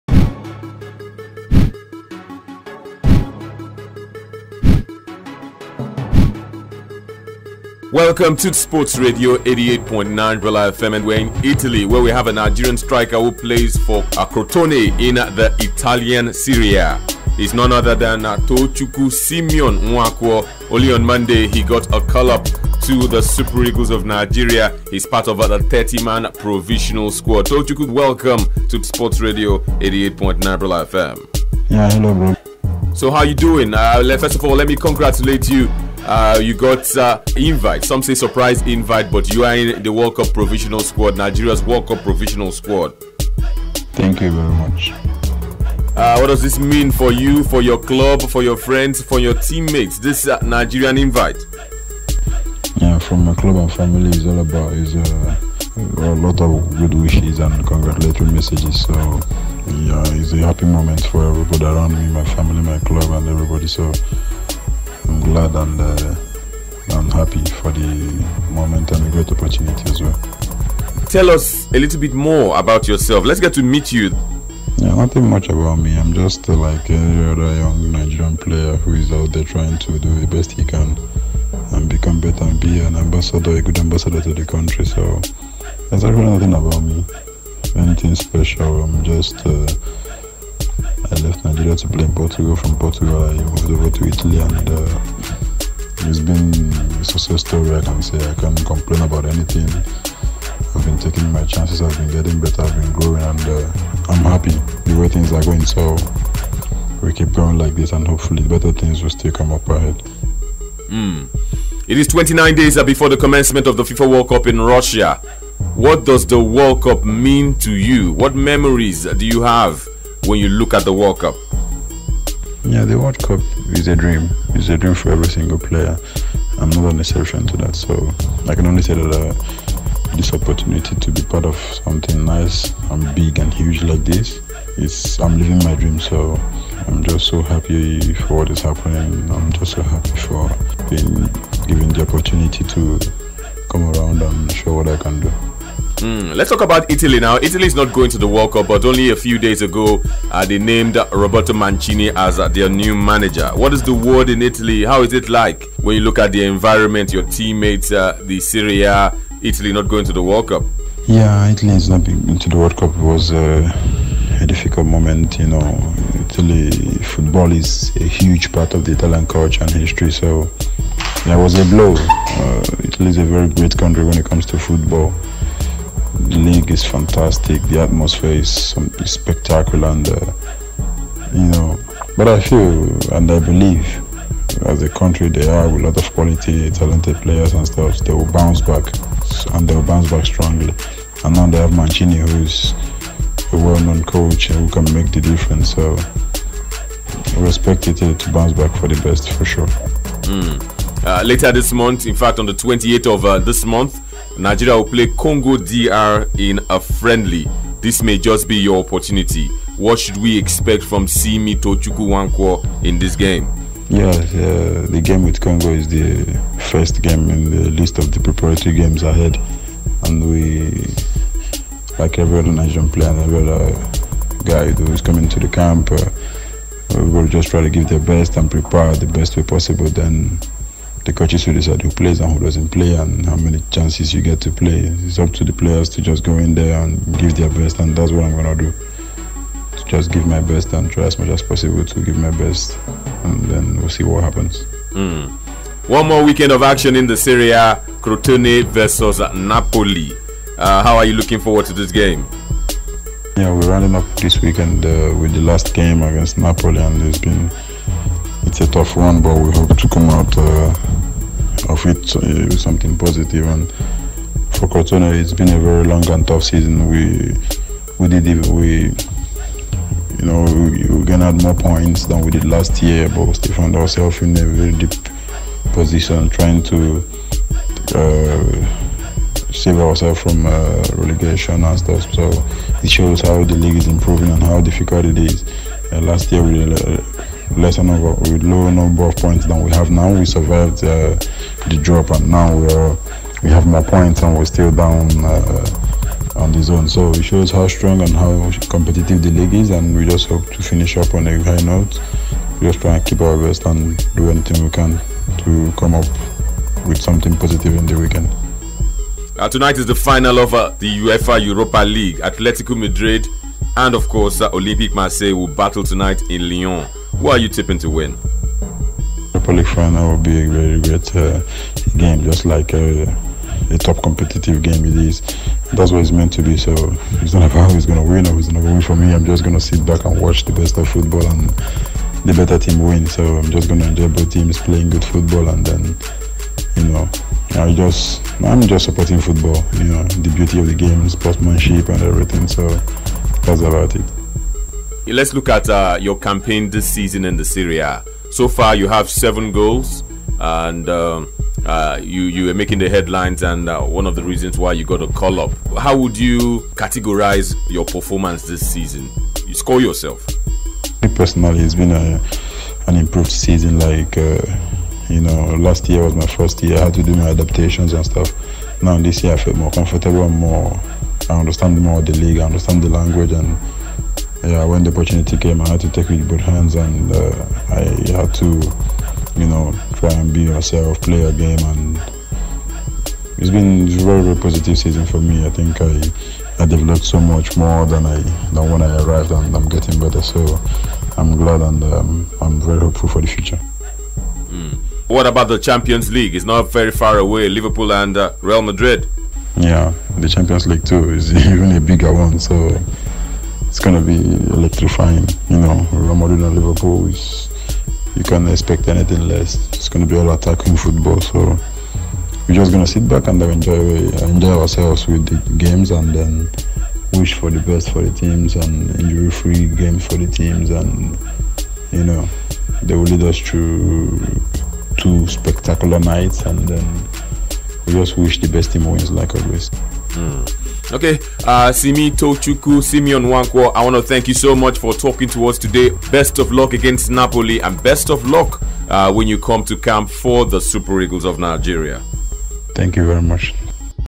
INTERVIEW SEGMENT WITH As Crotone of Italy rookie, Simeon Tochukwu Nwankwo (Simy)
INTERVIEW SEGMENT WITH  As Crotone of Italy rookie, Simeon Tochukwu Nwankwo (Simy) by Anthony Bekederemo